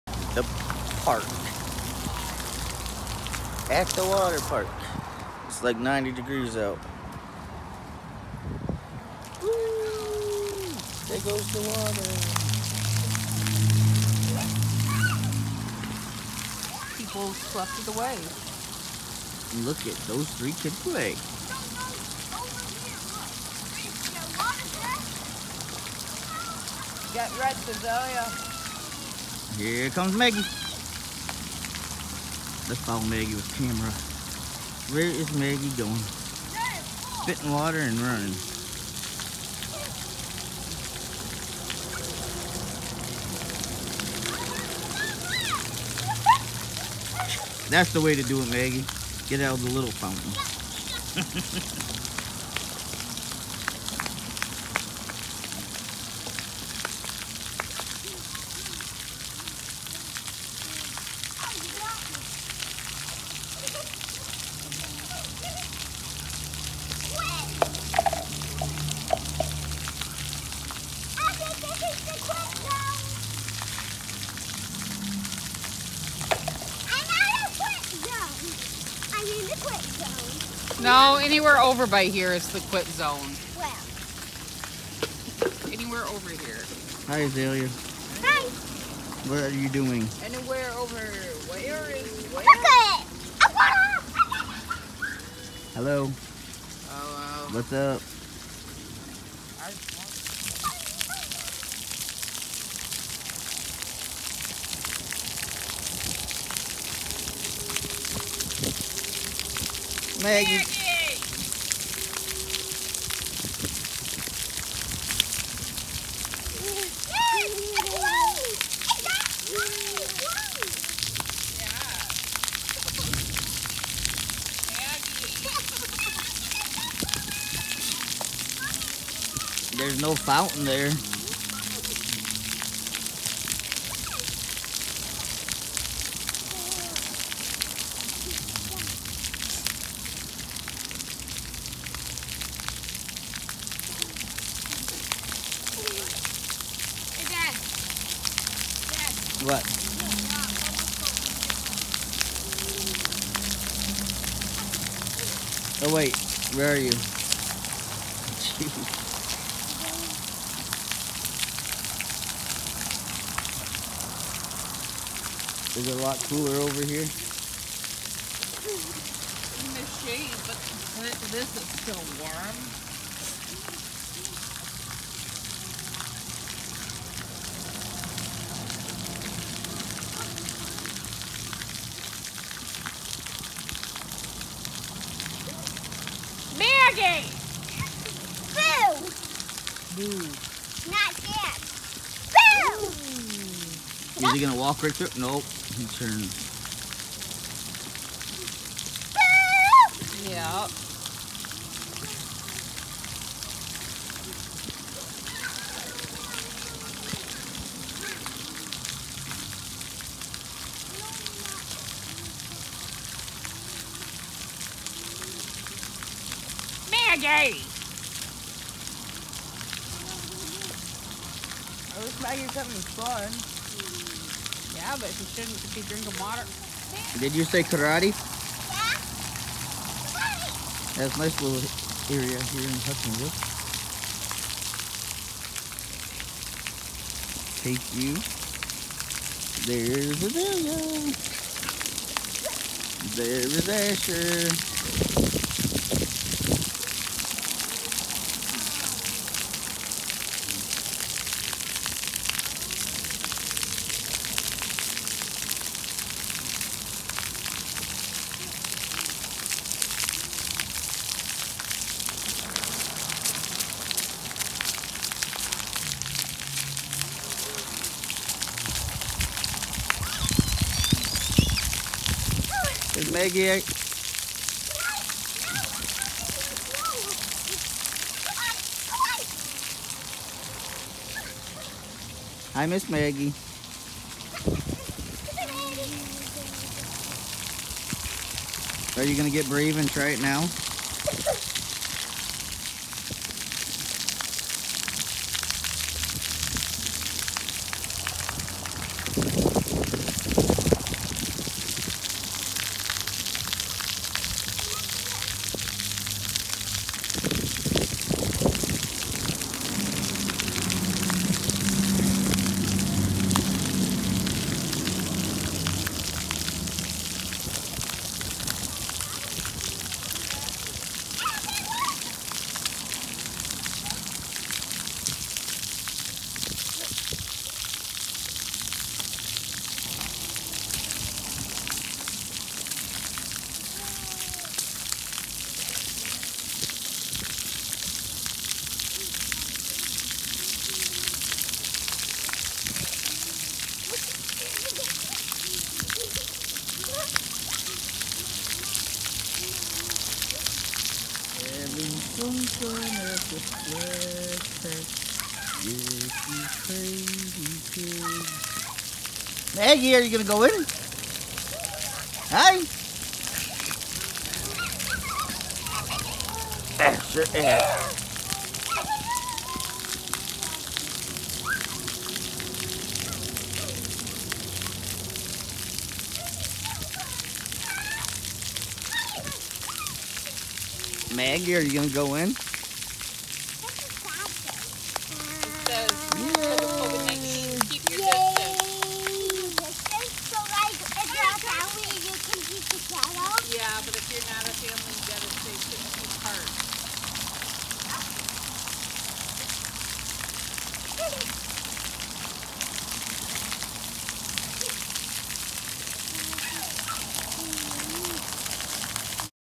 Summer time fun Terra square!
Out with the family to cool off at the water pad at Terra square ! Making memories with the family !